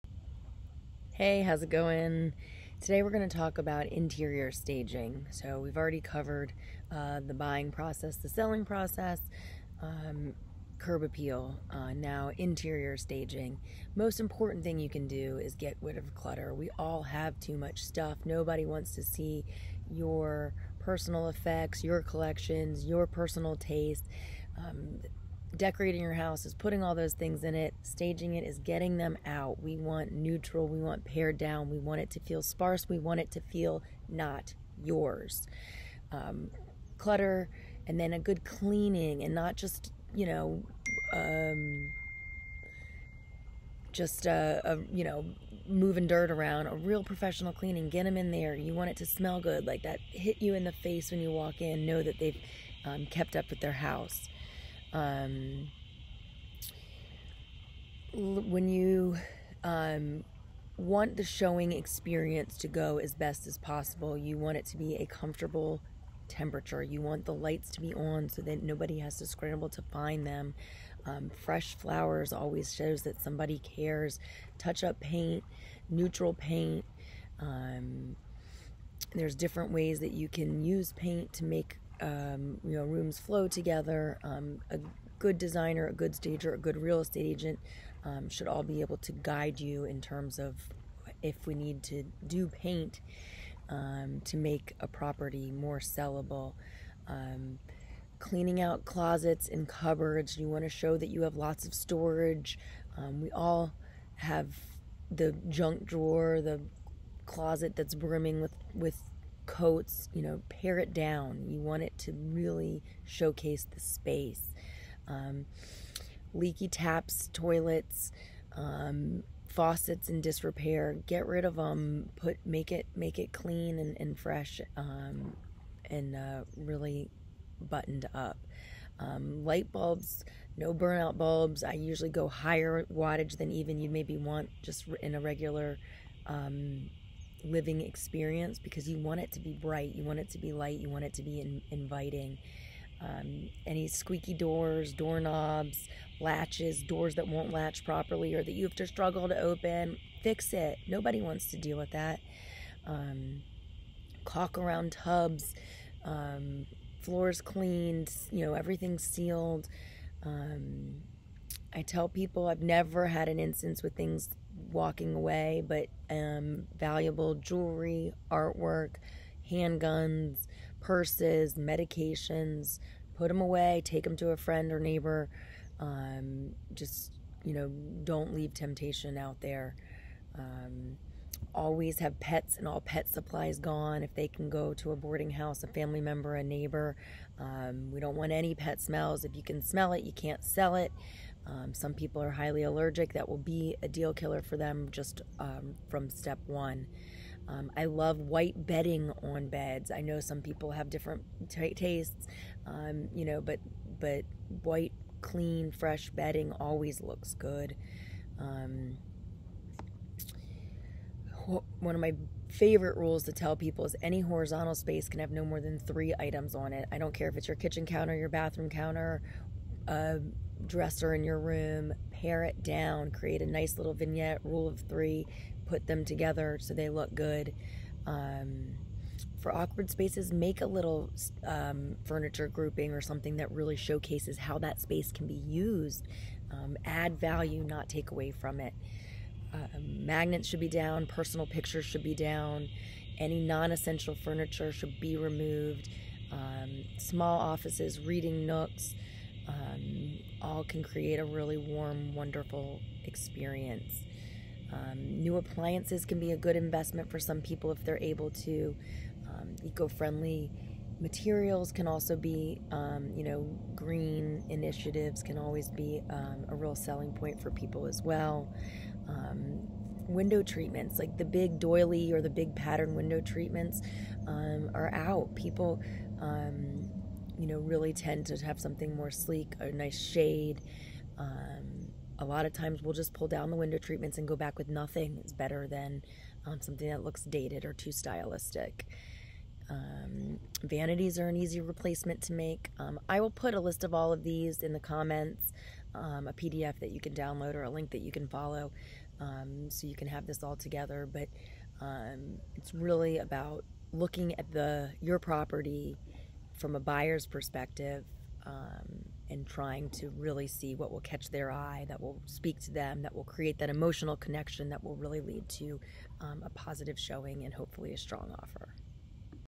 Interior Staging
Kathryn shares practical tips on decluttering, neutralizing color palettes, and strategically placing furniture to create an inviting atmosphere that appeals to a wide range of tastes. Discover how a well-staged home can not only attract more potential buyers but also increase its perceived value, leading to a quicker and more lucrative sale.